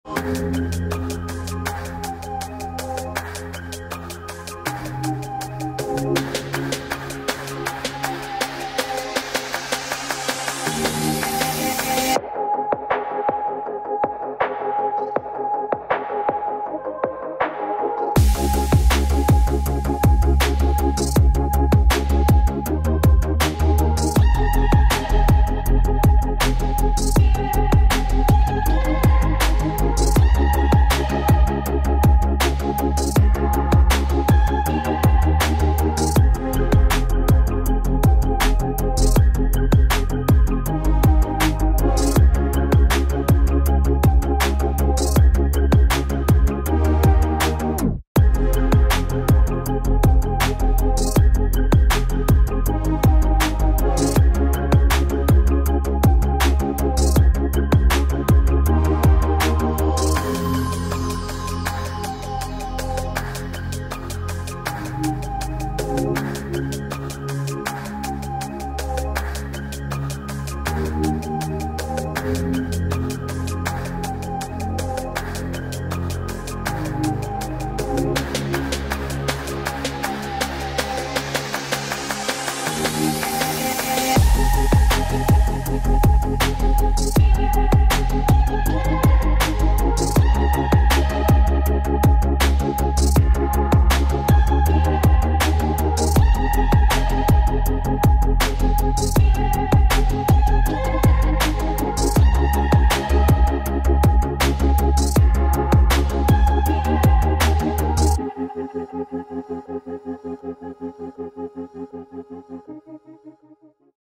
2024 BMW 7 SERIES Automated Parking Demonstration
Park the 2023 BMW 7 Series remotely in or out while standing outside with the My BMW App on iPhone to control the Automatic Parking Assistant or Automatic Maneuver Assistant from the start or to take over the automatic parking process.

✅ Source: BMW